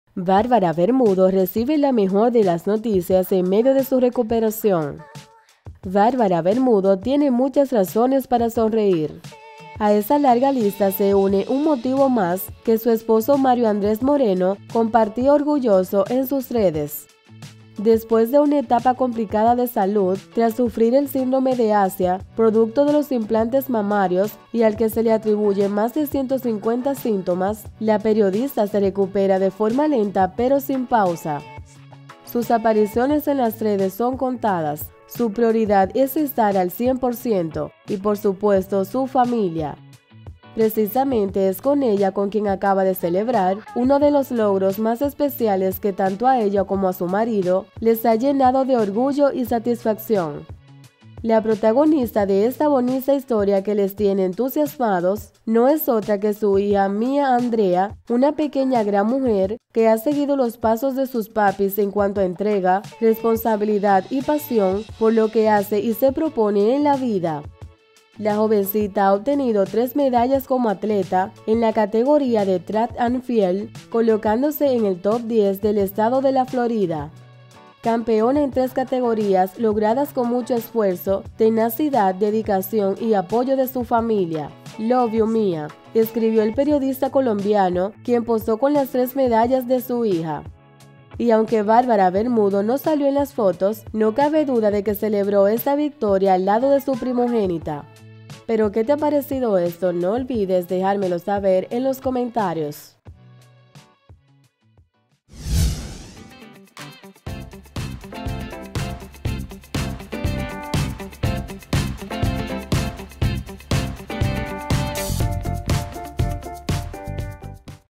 😱😍Bárbara Bermudo RECIBE la MEJOR de las NOTICIAS en medio de su RECUPERACIÓN 🚨
😱😍Bárbara Bermudo RECIBE la MEJOR de las NOTICIAS en medio de su RECUPERACIÓN 🚨 La periodista y su marido, Mario Andrés Moreno, celebraron una gran noticia en familia.